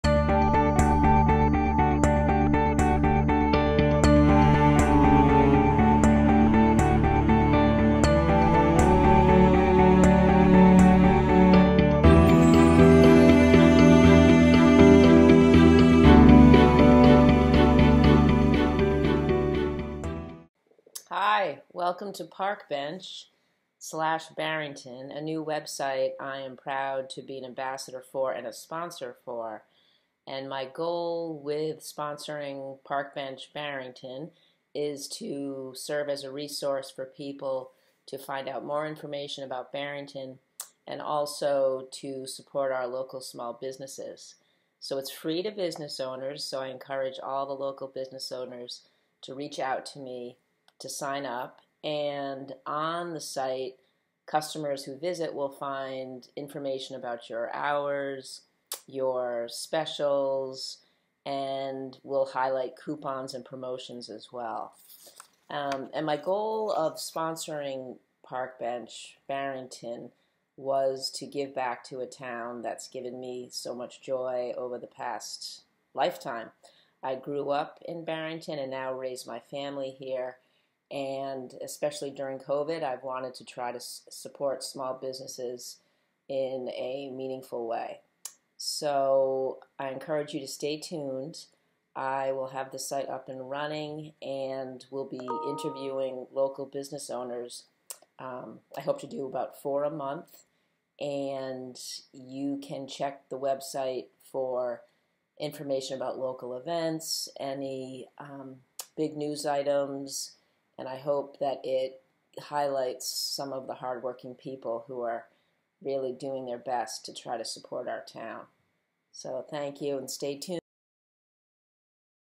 Welcome to Parkbench- Barrington, RI!
Welcome to Parkbench.com/Barrington-RI!

I am thrilled to share a new local website I am proud to sponsor and be an ambassador for!

After a recent bout with some health challenges I decided to finally go for it and sponsor a website as a means of giving back to a town and community I adore.

The site is designed to be a resource for all things Barrington while also highlighting our small business owners who are the heart beats behind the 02806.

It is free to business owners and my goal is to present to the world and to our Barrington community all the incredible souls giving so much of themselves through their courageous small business endeavors.   I will showcase the site after my first interview and encourage all those small business owners in Barrington to message me to sign up for an interview! Here's to all who make this town such a wonderful place to live, work and "play in."   I look forward to seeing you all out and about dining, shopping and enjoying everything local!  Take Care Barrington !